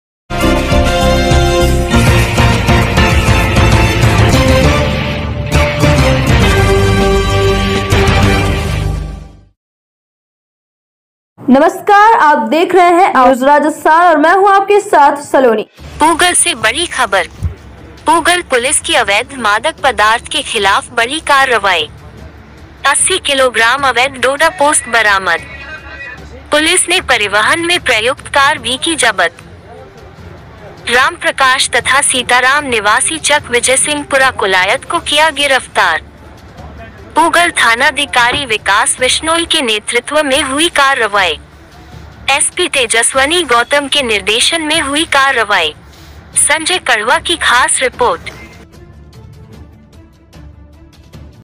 पूगल पुलिस की अवैध मादक पदार्थ के खिलाफ बड़ी कार्रवाई 80 किलोग्राम अवैध डोडा पोस्त बरामद,
पूगल पुलिस की अवैध मादक पदार्थ के खिलाफ बड़ी कार्रवाई

80 किलोग्राम अवैध डोडा पोस्त बरामद,

पुलिस ने परिवहन में प्रयुक्त कार भी की जब्त,

 रामप्रकाश तथा सीताराम निवासी चक विजयसिंहपुरा कोलायत को किया गिरफ्तार,

 पूगल थानाधिकारी विकास विश्नोई के नेतृत्व में हुई कार्रवाई,

 SP तेजस्वनी गौतम के निर्देशन में हुई कार्रवाई

संजय कड़वा की खास रिपोर्ट